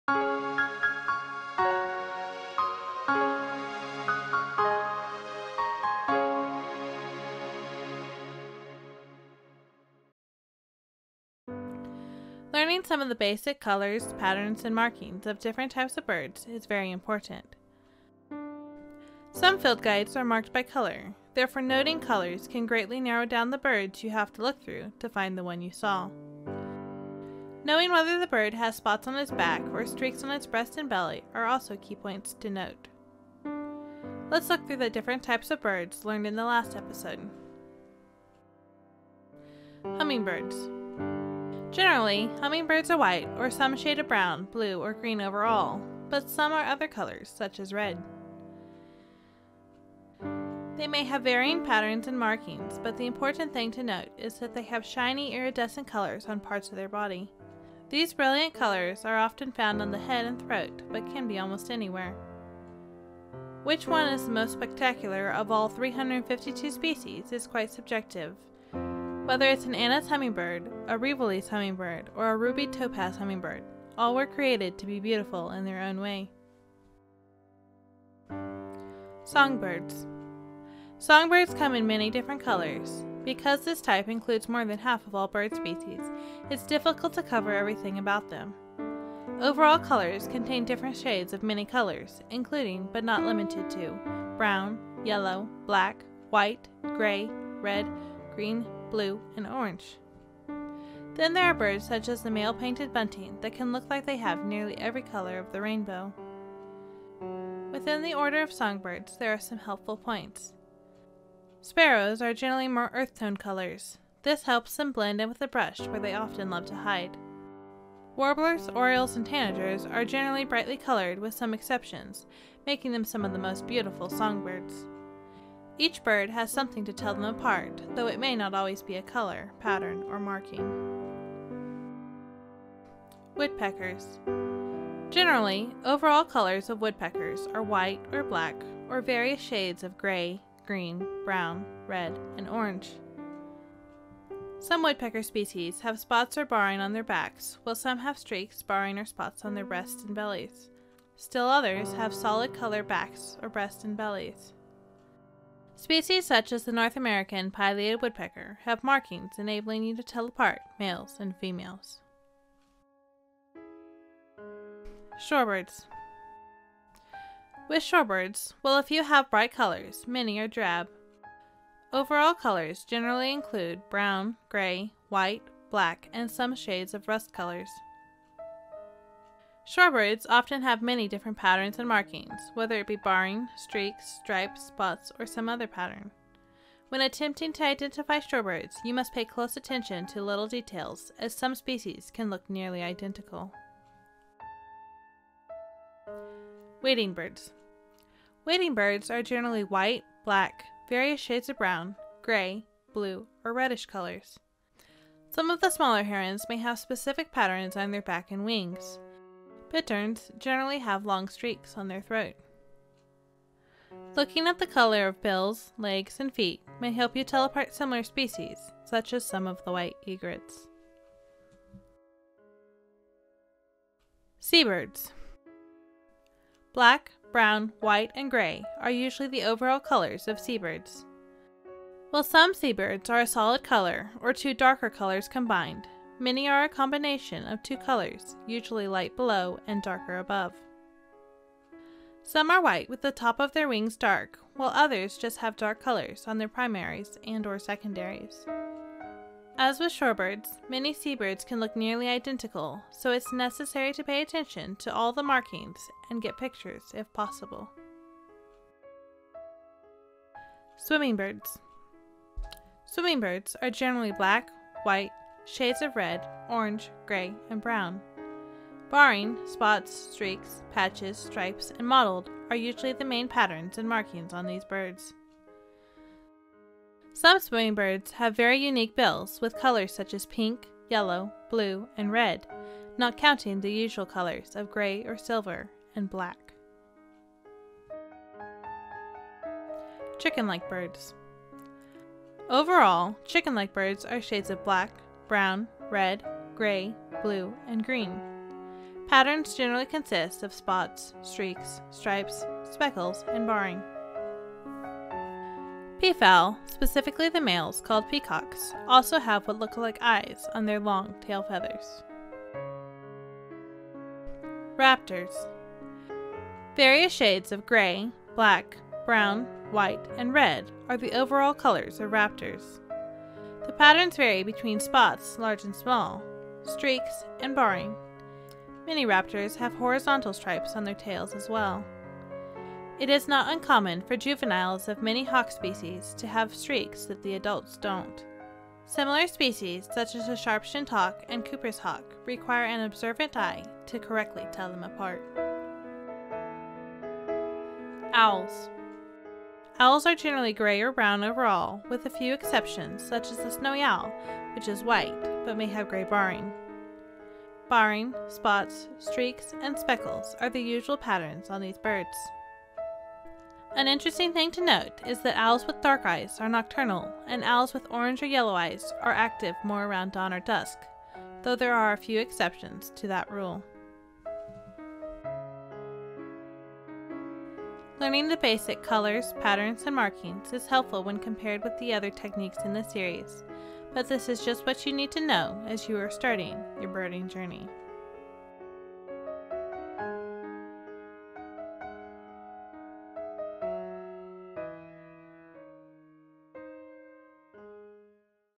Birding by Colors, Patterns, and Markings | 5 Key Tips of Bird Identification Ep. 3
What was the color of the bird you saw? Learn the overall colors of different types of birds in this video.